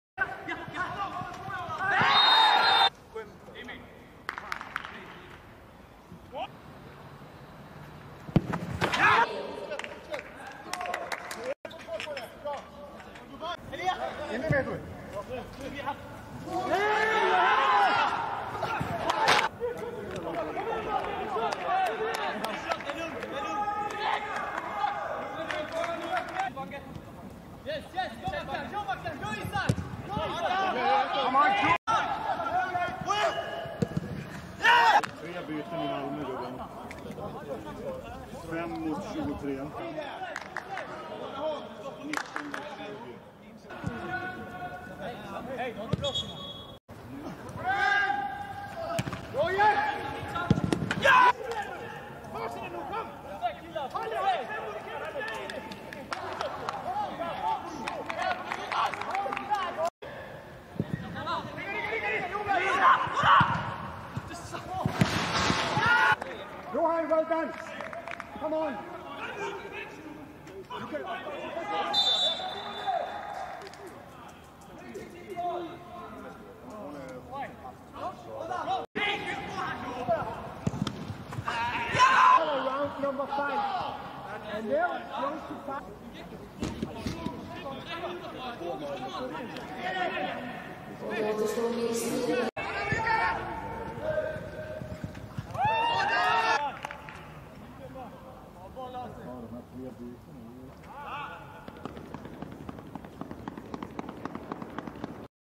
IK Sirius Malmö FF 2-5 Video granskning
SVERIGE

Allsvenskan
IK Sirius  Malmö FF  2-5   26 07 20

Straffspark - 1:0 - 18'    Saeid M. (Straff), IK Sirius
Självmål - 1:1 - 51'    Andersson E. (Självmål), Malmö FF
Mål - 2:1 - 56'   Vecchia S. (Saeid M.), IK Sirius
Mål - 2:2 - 65'   Toivonen O., Malmö FF
Mål - 2:3 - 85'   Thelin I. (Toivonen O.), Malmö FF
Mål - 2:4 - 90'   Berget J. (Larsson E.), Malmö FF
Mål - 2:5 - 90+4'   Rieks S. (Thelin I.), Malmö FF